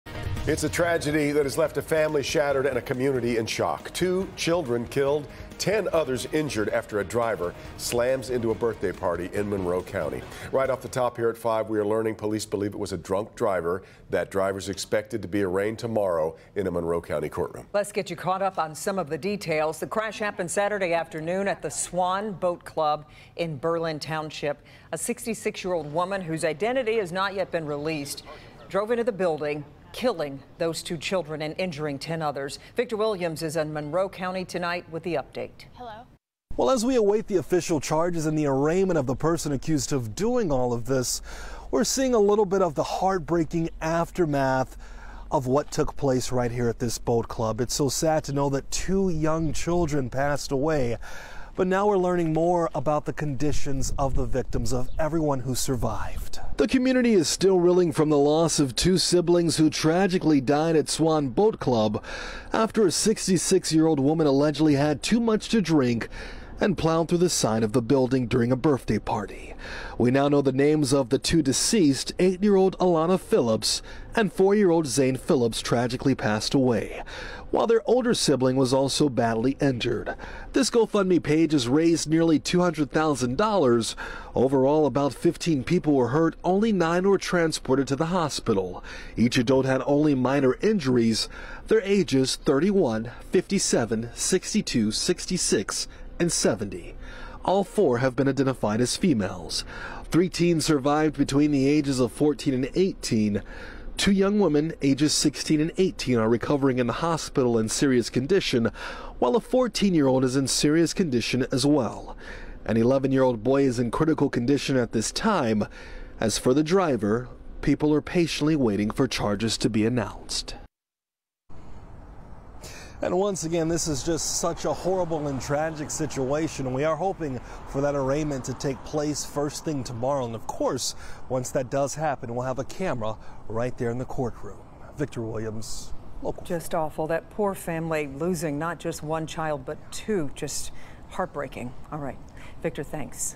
Monroe County police share ages of those hurt in deadly birthday party crash
Several children and adults were injured Saturday when a suspected drunk driver crashed into a child’s birthday party in Monroe County, killing two children.